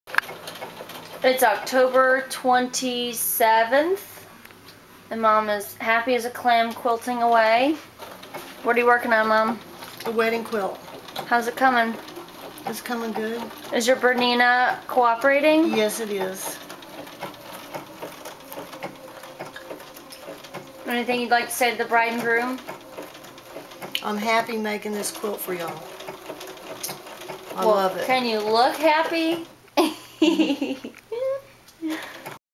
Happy Wedding Quilt Making
Mom was working on a wedding quilt for Chad and Angee and I thought it would be fun to capture her in progress.